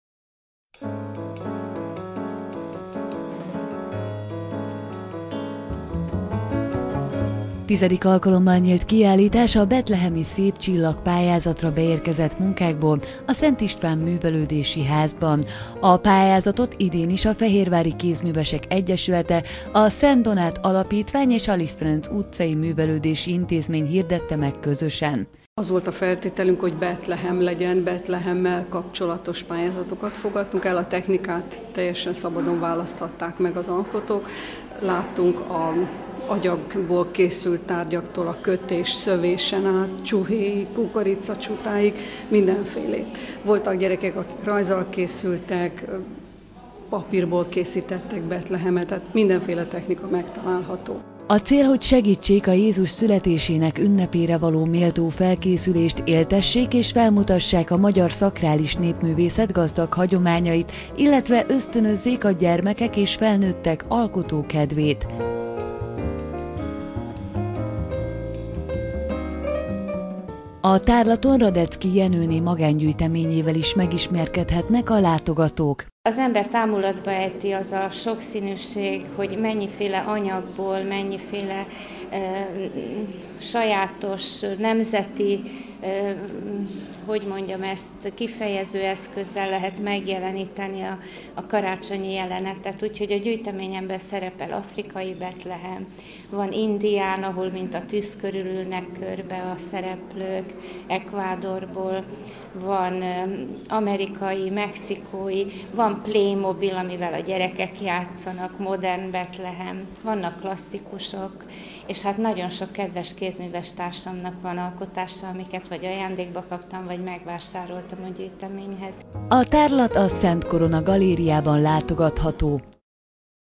Betlehemi szép csillag
Szombaton az adventi készülődés jegyében megnyílt a Betlehemi szép csillag kiállítás. A Szent István Művelődési Házba az idei pályázatra is sok szép alkotás érkezett. Ezek között a többféle kézműves technikával készült Betlehemek között barangolhatnak most a látogatók.